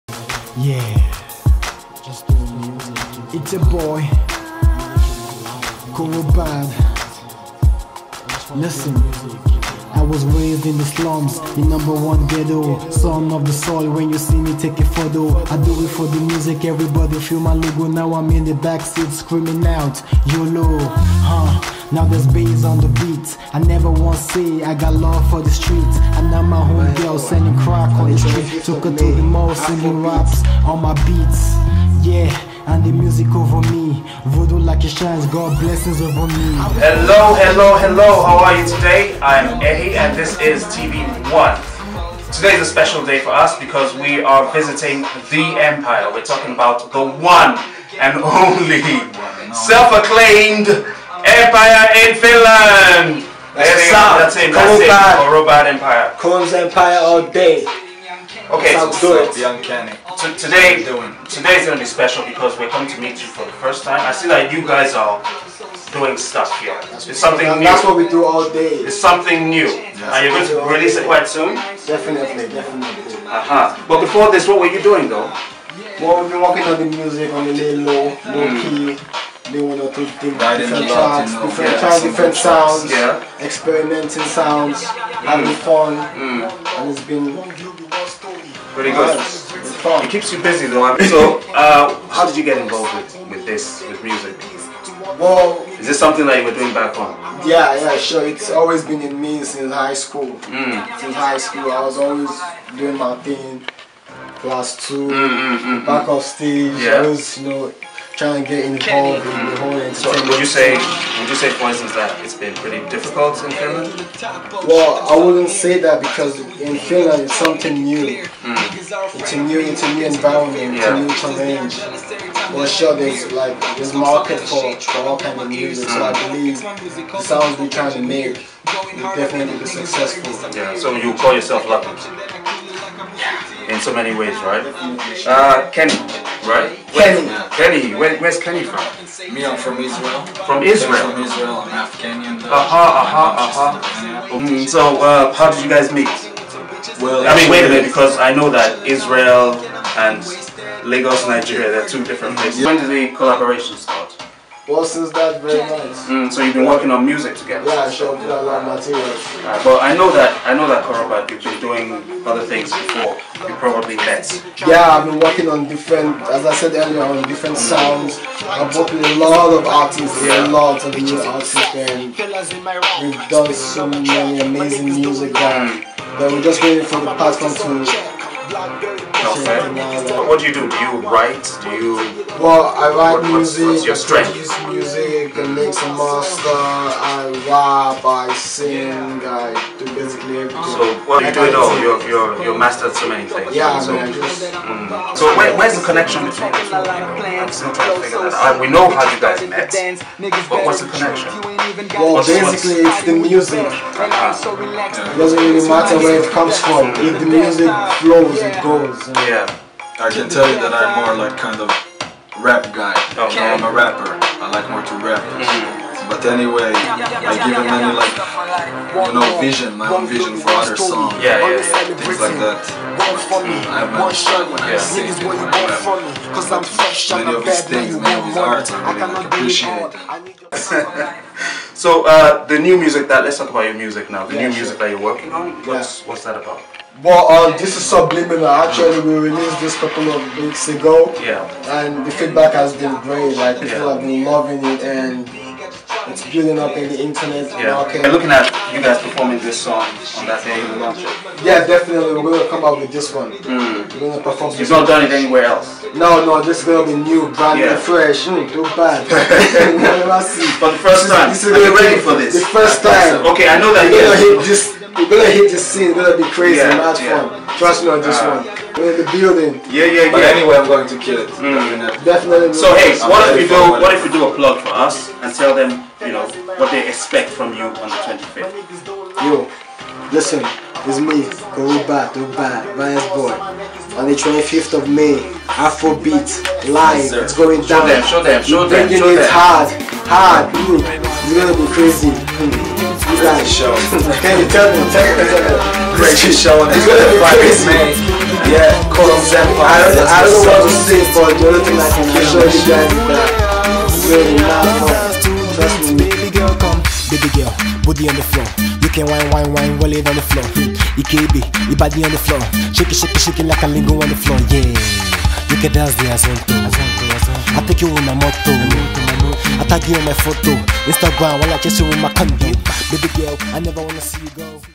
Afrobeat FINLAND visits Korobad's Koroms Empire
Exclusive interview
Afrobeat Finland presents Korobad